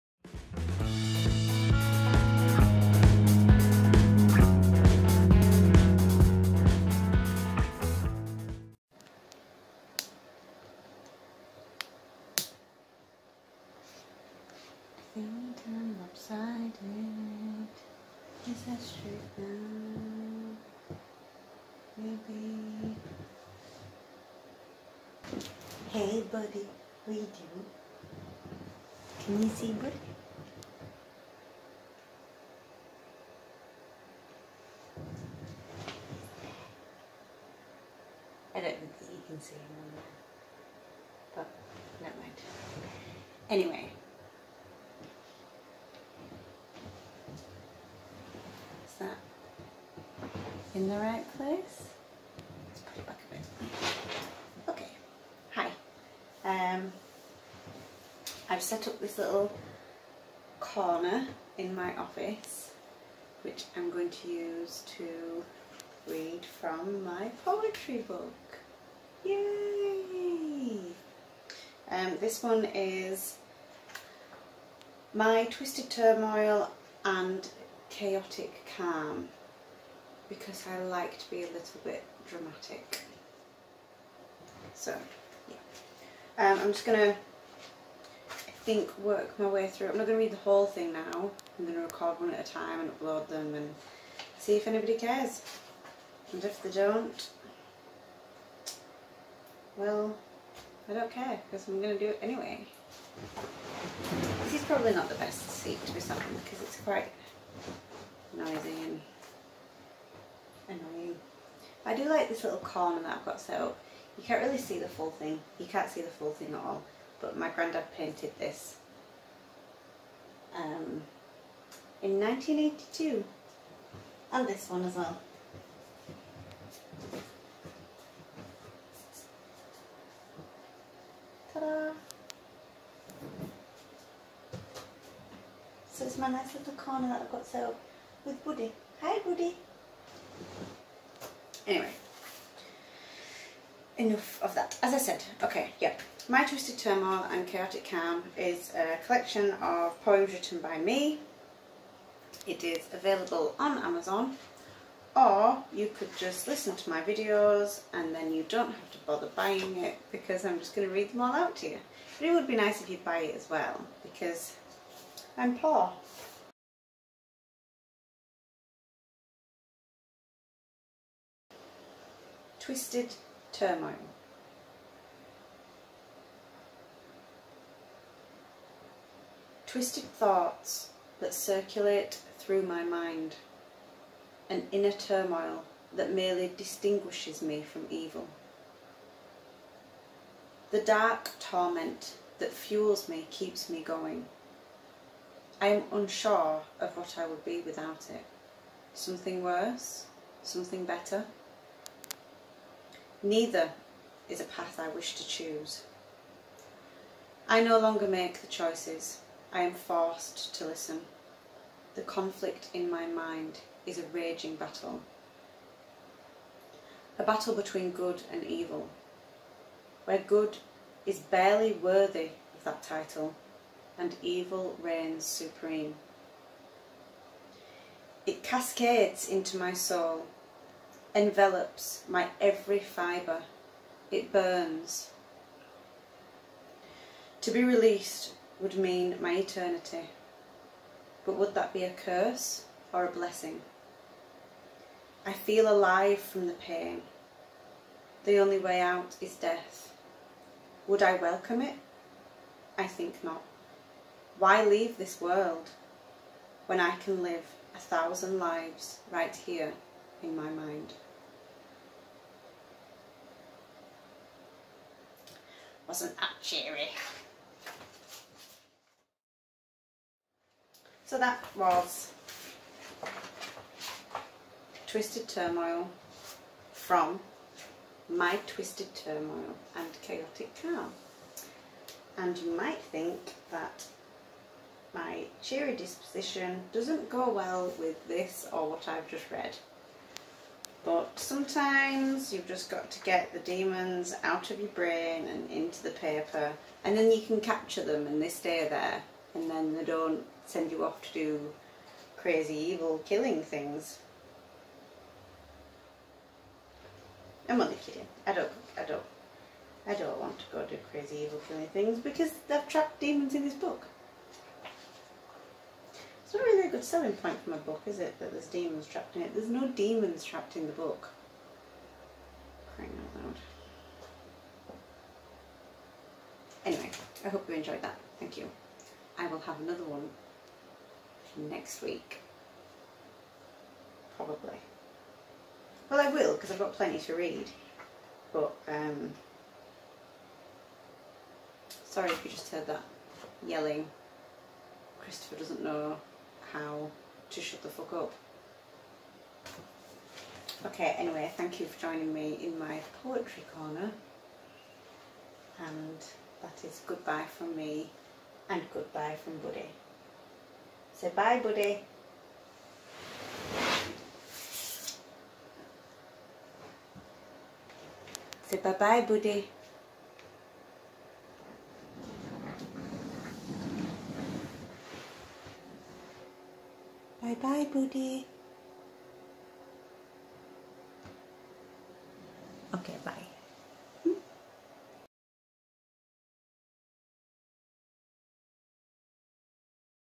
Twisted Turmoil - A poem by Kate Hames
I'm sharing my poetry with you, potentially on a weekly basis...
Most of my writing comes from a dark time in my life, I only seem to be able to write when I'm feeling low, I suppose that's where the emotion comes from in songs, poetry, etc.

Thank you, you beautiful humans x